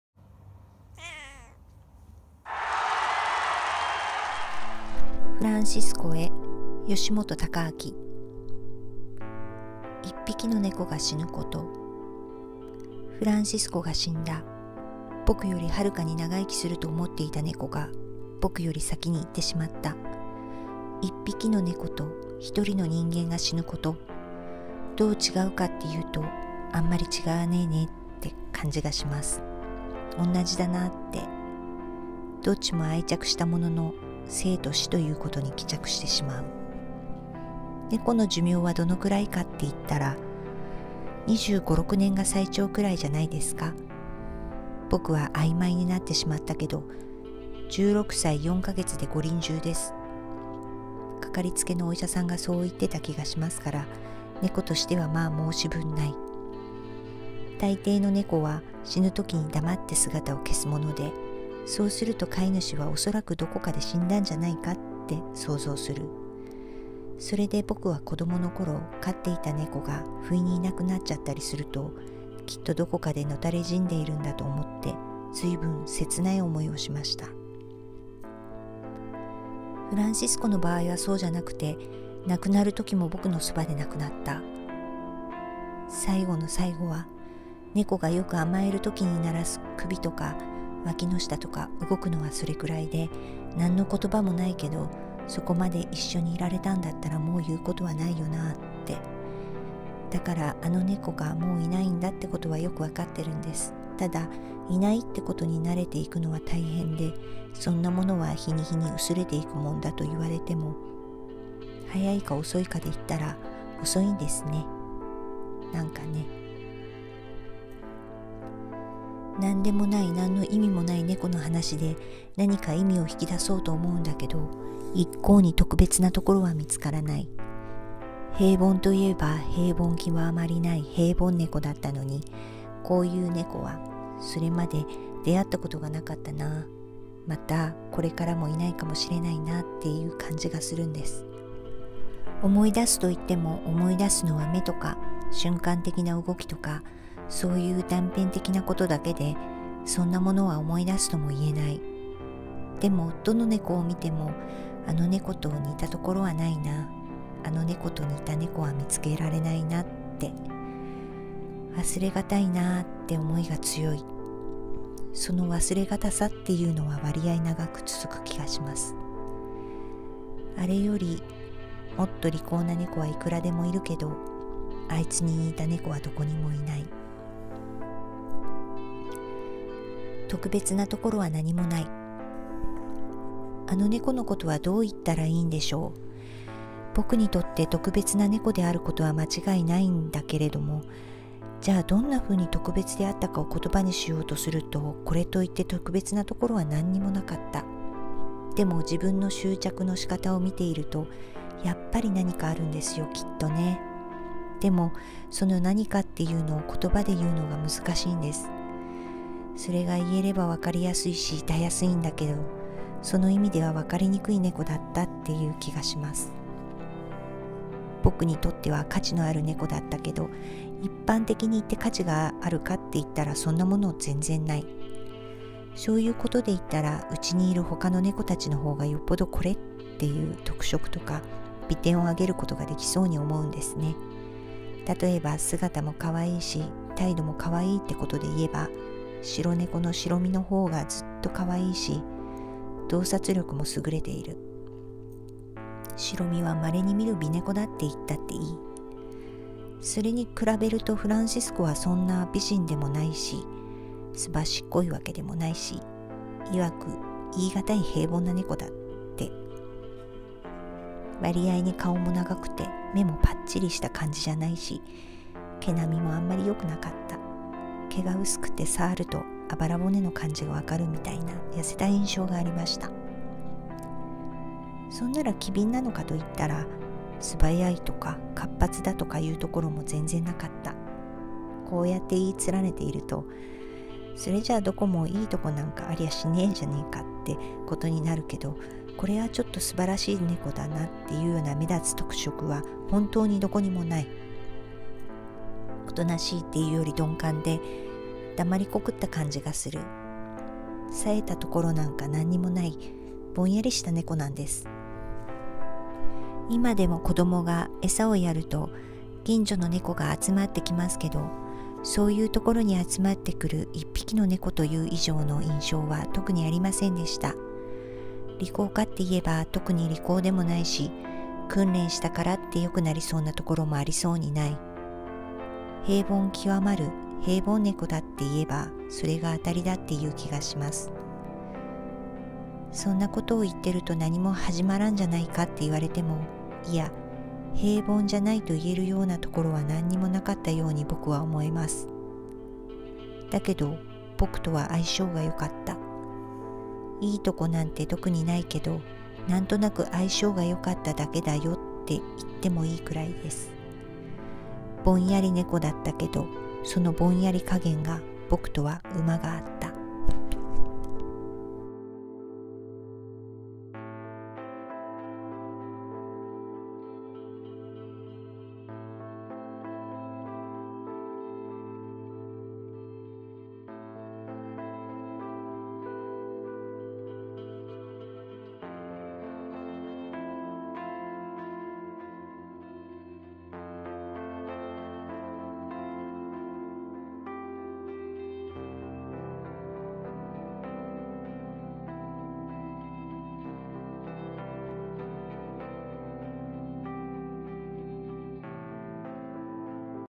【朗読】フランシス子へ 吉本 隆明 Reading Takaaki Yoshimoto
思想界の巨人の晩年お気に入りだった猫への思い。
2小節までで本編には続きがあります。
フランシス子が死んで一年足らずで吉本氏も逝去。
深く読むと涙がでます

秋の黄昏時にでもゆっくりいかがでしょうか